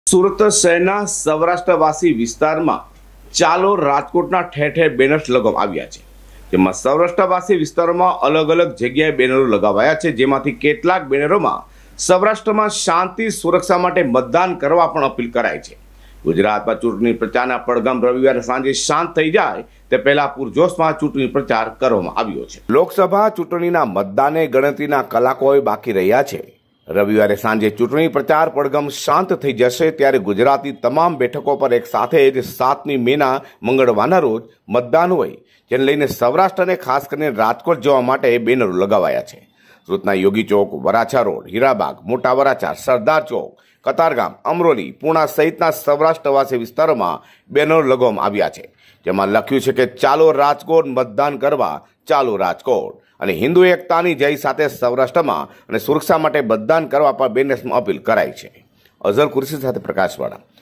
સુરતમાં ચાલો રાજકોટના લાગ્યા ઠેર ઠેર બેનરો
સુરતમાં સૌરાષ્ટ્રવાસી વિસ્તારોમાં અલગ અલગ જગ્યાએ બેનરો લાગ્યાં